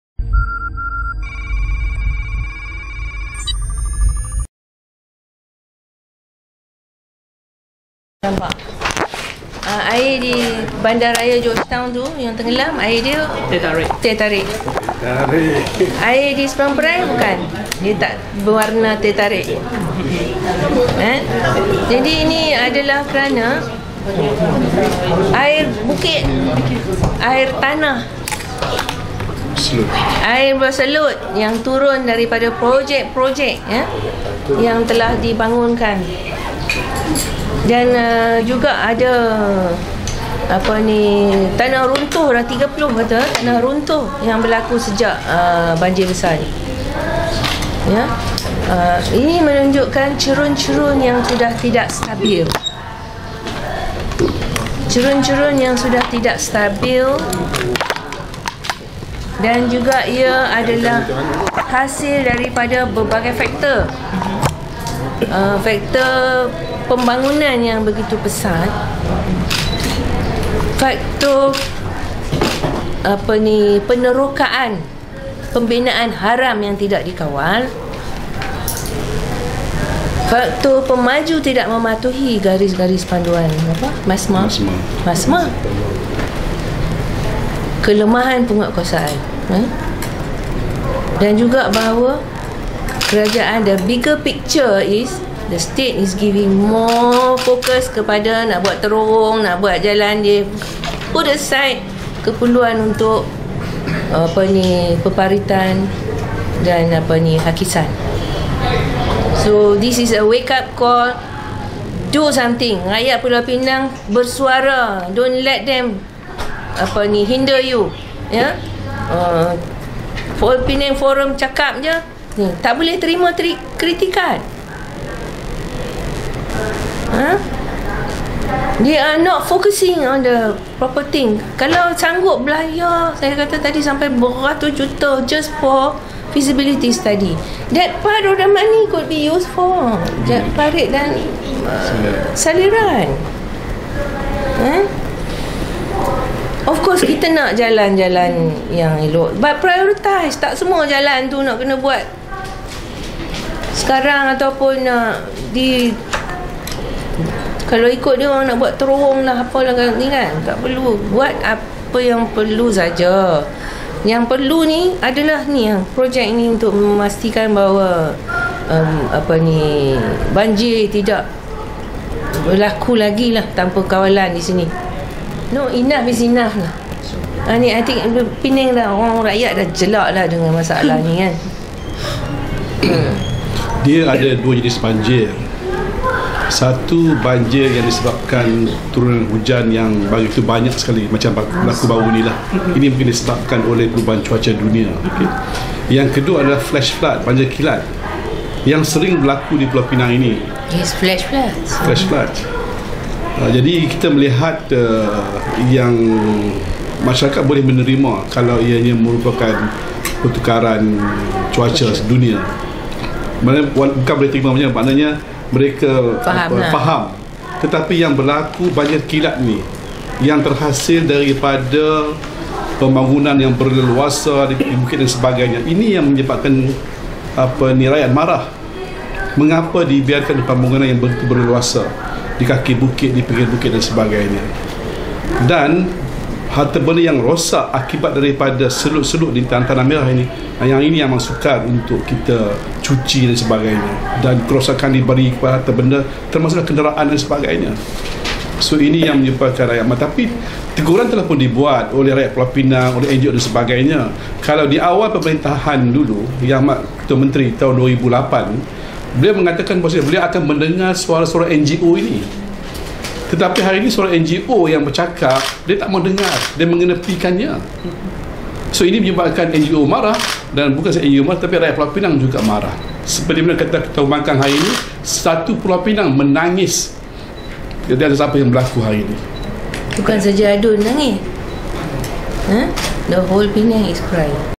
Rakyat Pulau Pinang Sudah Jelak dengan Masalah Banjir & Tanah Runtuh
8 Nov 2017 - Ketua Pembangkang , Adun Telok Air Tawar menempelak Kerajaan DAP Pulau Pinang masih tidak menyelesaikan masalah Banjir & Tanah Runtuh di negeri ini